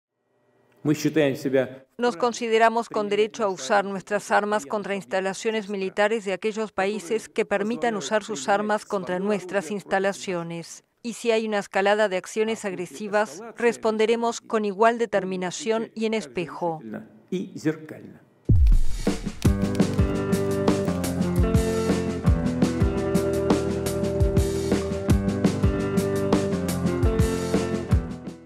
Putin dice que Rusia tiene "derecho" a atacar a países que proporcionan armas a Ucrania
El presidente ruso, Vladímir Putin, afirmó el jueves que Moscú se arroga el derecho de atacar objetivos militares de países que procuren armas a Ucrania para bombardear territorio ruso.